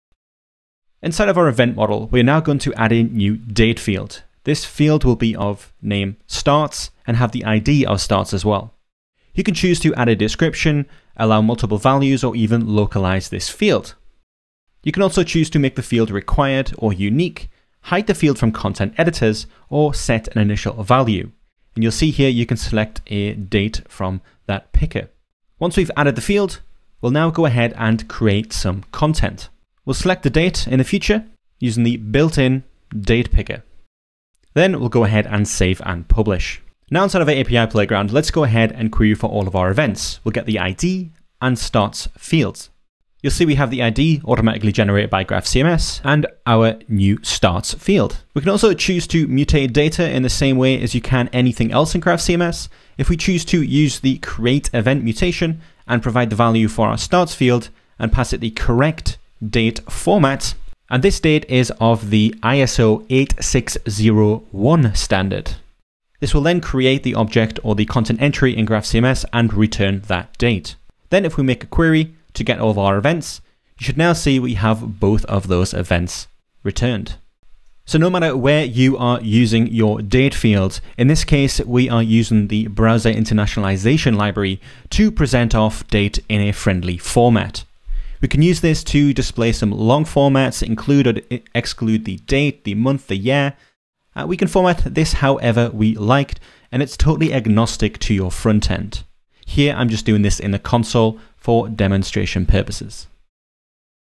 Working with the Date field | How to Hygraph
The Date field type adheres to ISO 8601 standard. This means, October 7, 1989 is represented as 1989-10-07.